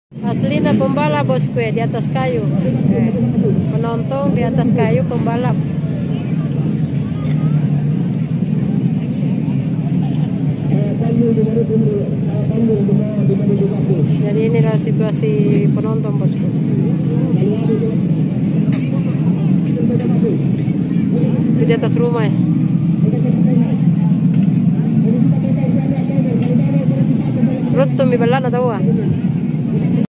KESERUAN PENONTON BALAPAN SAMPAI NAIK ATAP GENTENG PANJAT POHON DI SIRKUIT MIJEN KOTA SEMARANG
Keseruan balapan di SIRKUIT Mijen Kota Semarang penonton sampai memanjat pohon dan naik ke atap genteng